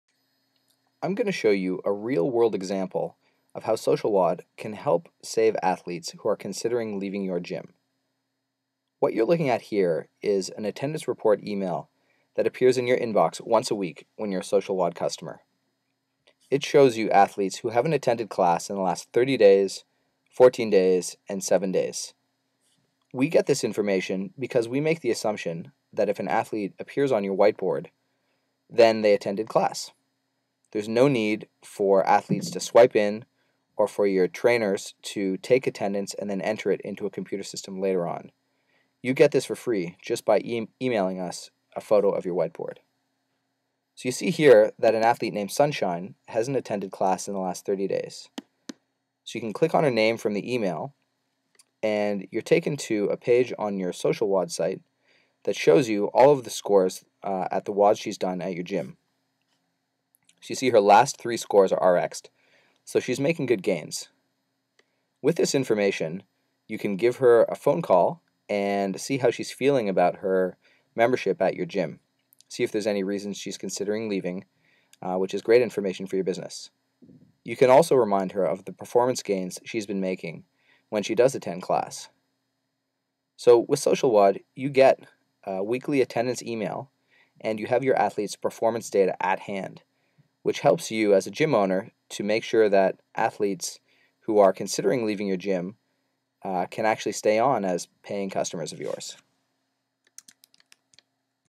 How SocialWOD can Help You Retain CrossFit Customers
See how SocialWOD can help you keep customers who may be thinking of leaving your gym.